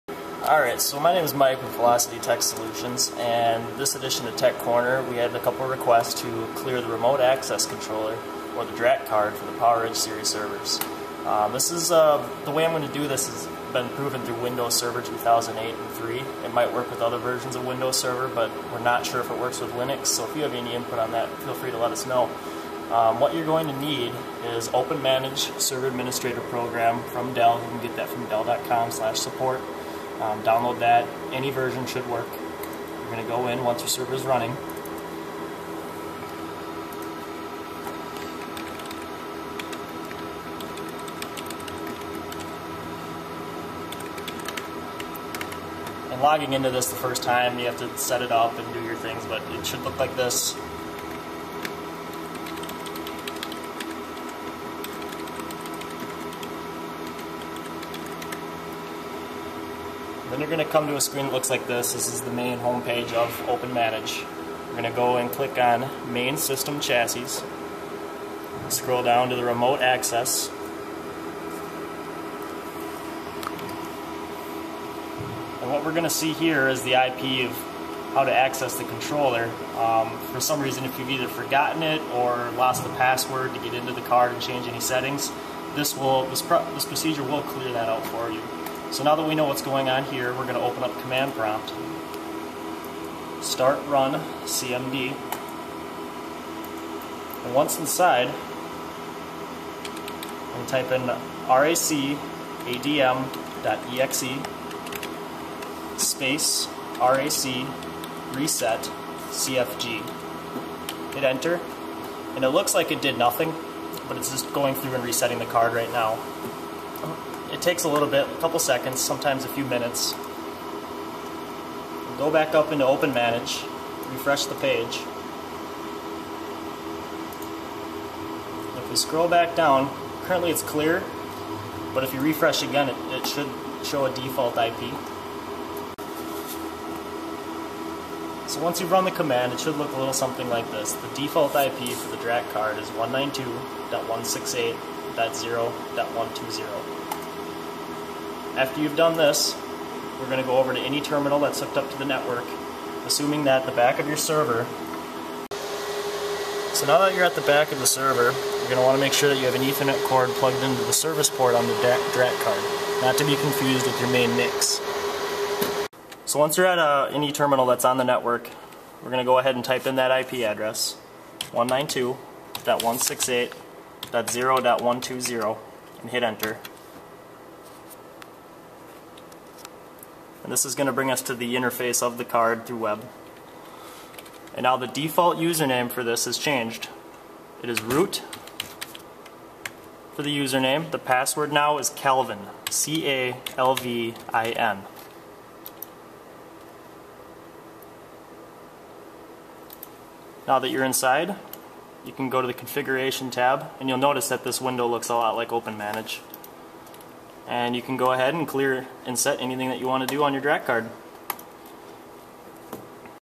Velocity Tech Solutions - Dell PowerEdge DRAC Reset Procedure
Velocity Tech Solutions presents the "Dell PowerEdge DRAC Reset Procedure" as a step-by-step tutorial.